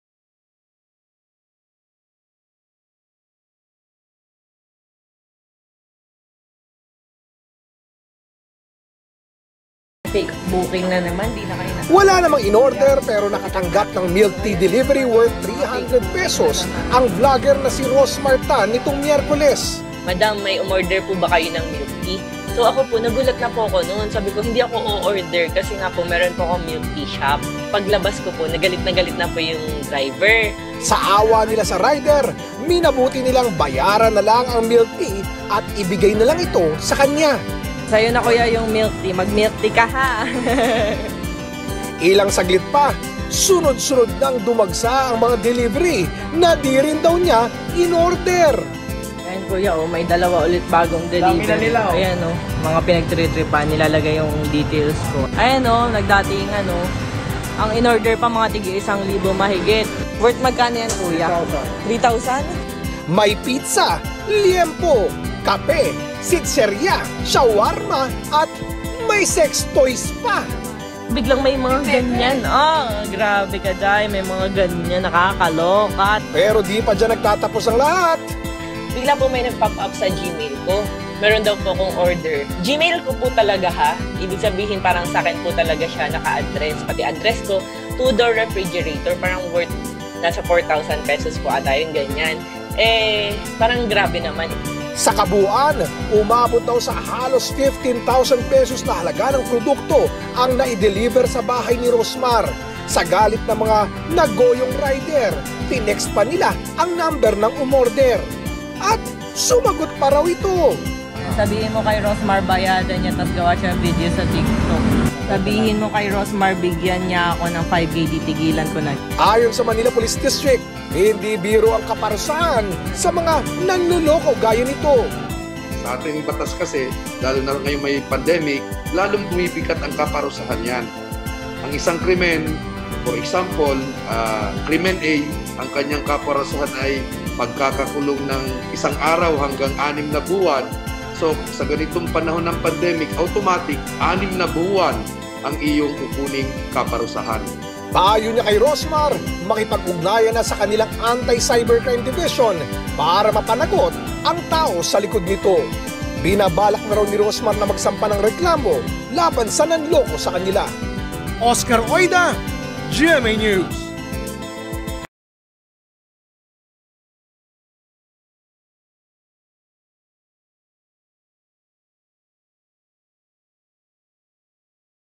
FAKE BOOKING, GMA NEWS REPORTS
Vlogger, pinadalhan at pinagbayad daw ng mga produktong aabot sa ₱15,000 kahit 'di daw niya inorder
Halos ₱15,000 halaga ng mga pagkain at produkto ang idineliver sa isang vlogger kahit hindi naman daw niya ito inorder! Ang biktima, tinangka pa raw kikilan ng P5,000 ng salarin! May report si OSCAR OIDA!